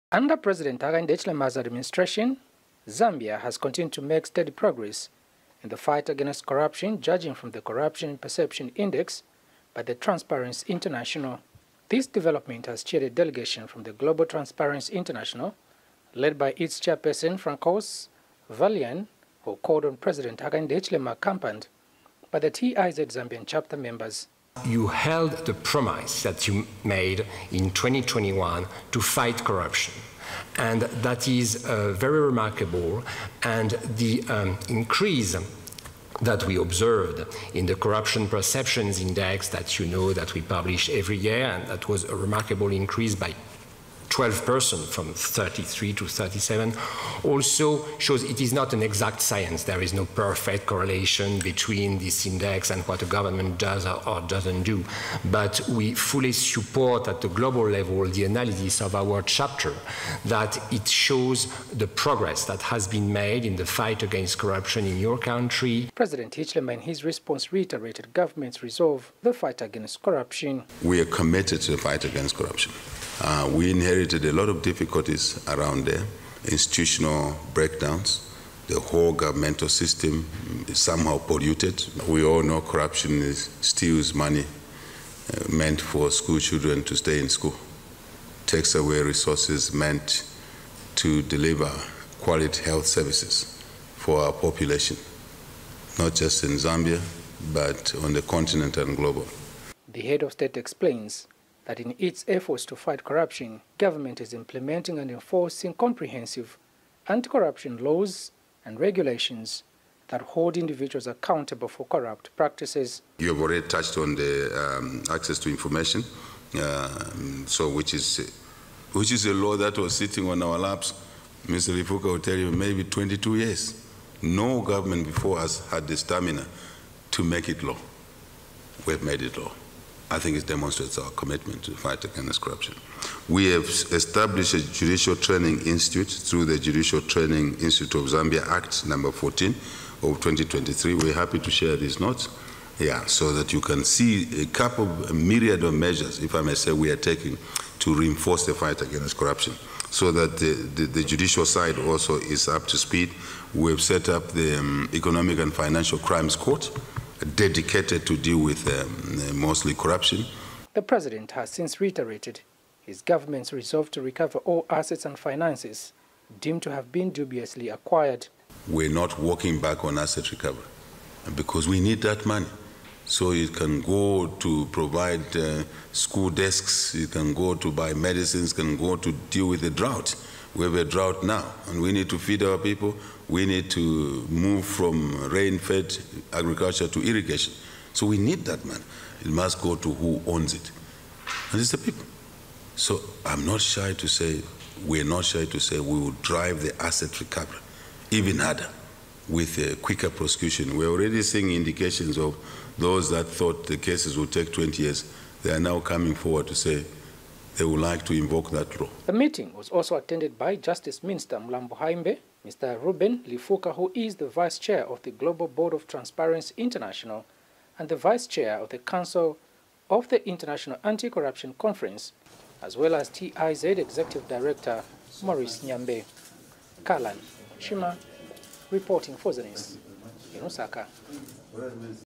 Govt resolved to fight corruption - HH
(ZANIS) President HAKAINDE HICHILEMA says his administration remains resolute in fighting entrenched and prevalent corruption in the country.

President HICHILEMA says in its efforts to fight corruption, the government is implementing and enforcing comprehensive anti-corruption laws and regulations that hold individuals accountable for corrupt practices.

He says corruption undermines economic development and poses a danger to democracy, Human Rights and social progress hence the need to root it out.

President HICHILEMA said this when a delegation from Global Transparency International led by its Chairperson FRANÇOIS VALERIAN  accompanied by Transparency International Zambia Chapter members called on him at State House.